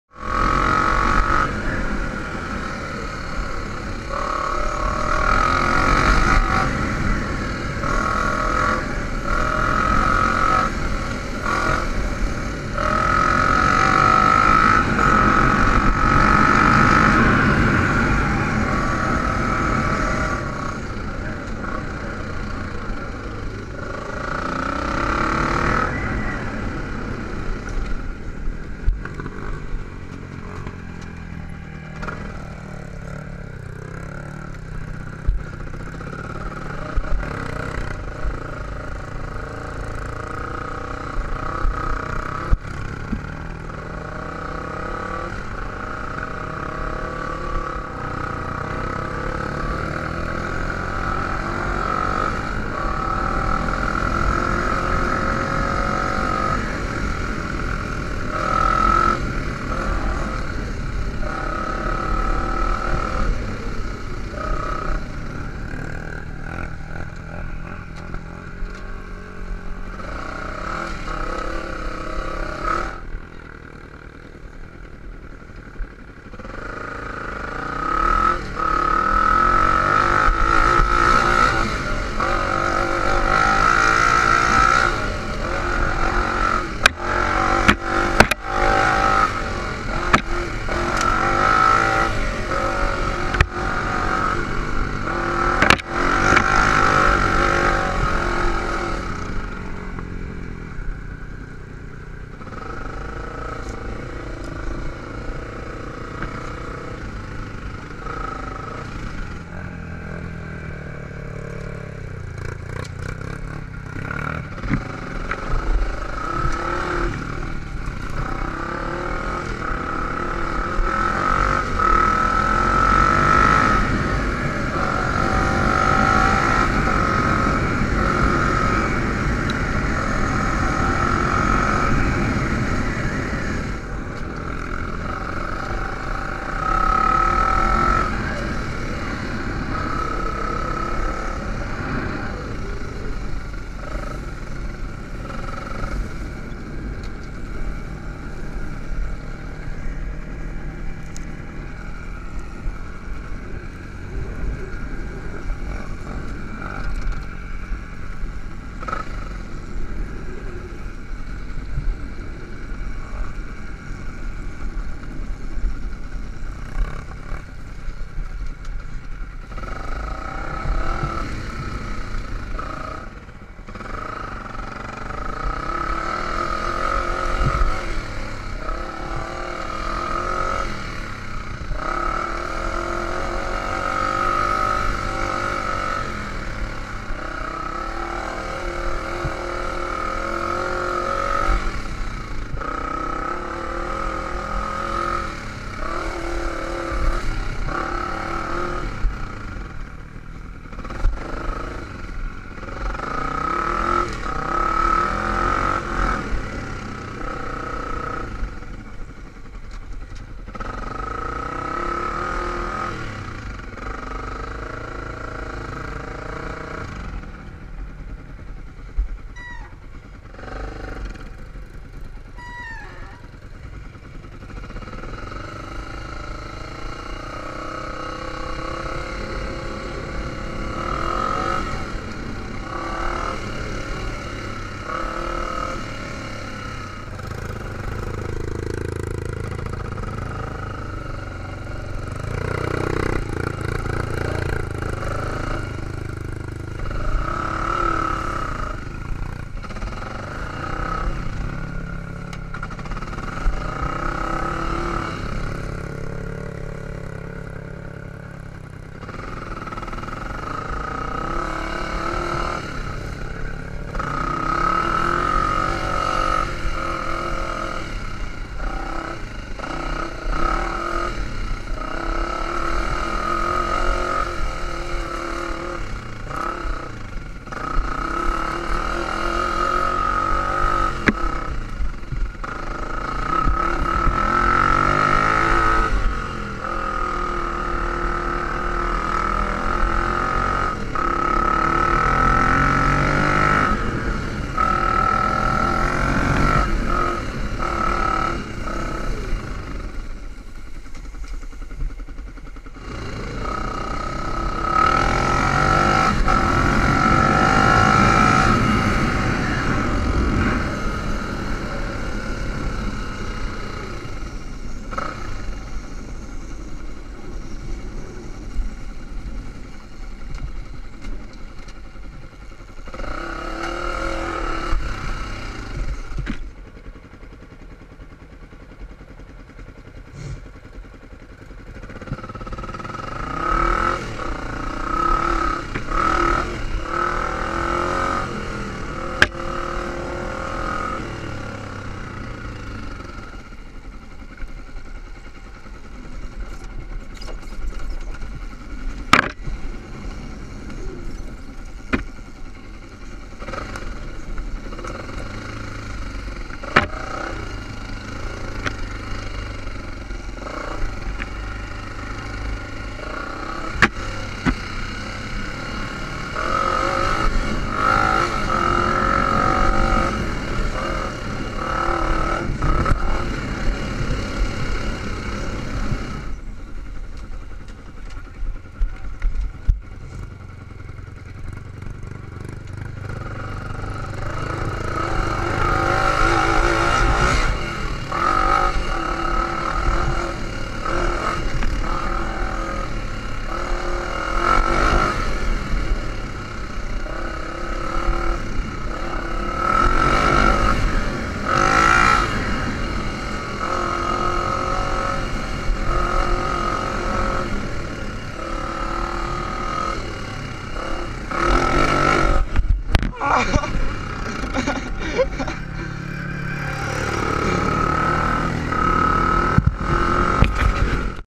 PICKING UP CHICKS ON DIRTBIKES AND QUADS!
ME AND A BUDDY GOING TO PICK UP A CHICK ON OUR QUADS AND DIRT BIKES NICE LITTLE RIDE WITH A HILLCLIMB AND RIDING THROUGH SKETCHY BULLRUSHES